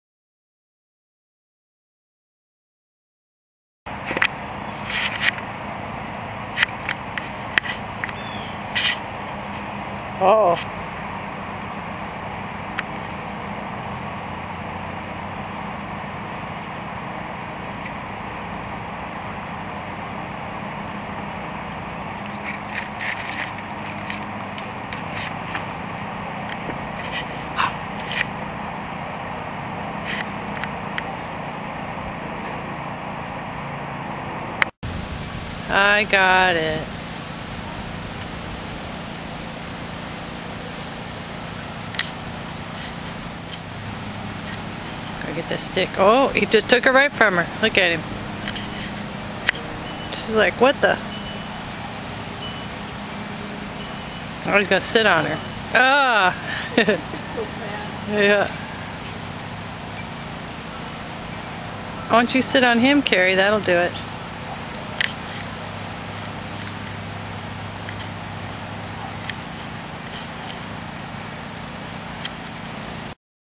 The Stick.wmv
Wheaten Terriers playing with a stick